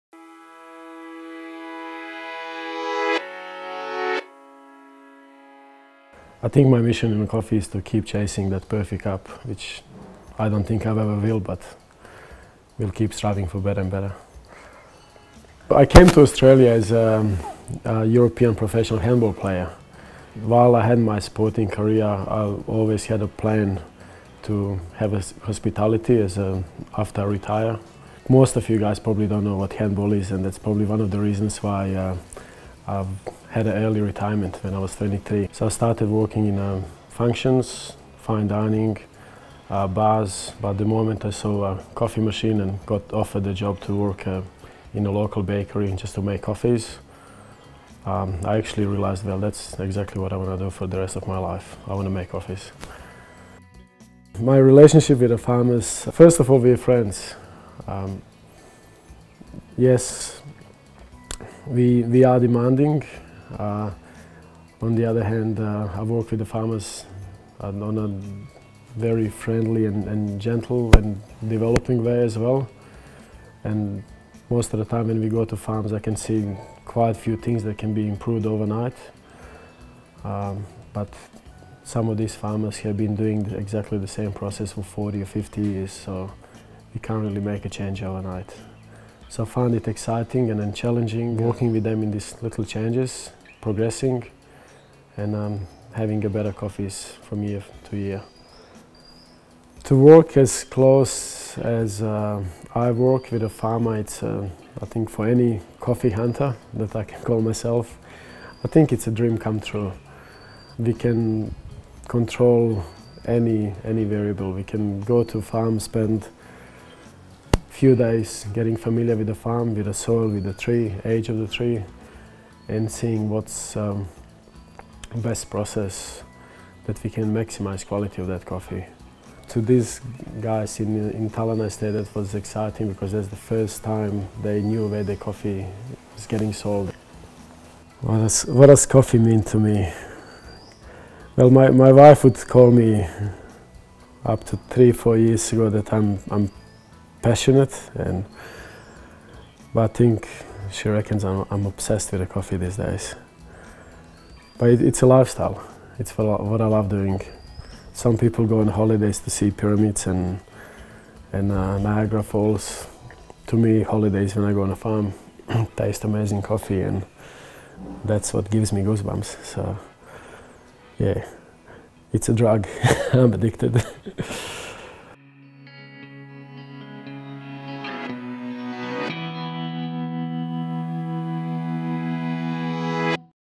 The Coffee Man - Sasa's Story
Smudge Publishing made this great little video on Sasa, founder of Project Origin.